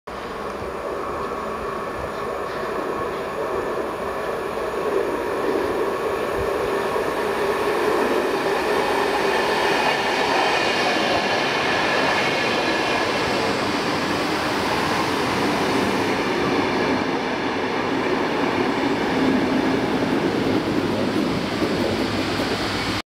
N juna saapuu Puistolaan
N juna Keravalta Helsinkiin saapuu Puistolan asemalle. Juna(t): Sm1, Sm2. Kuvattu: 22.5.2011. Paikka: Puistolan asema.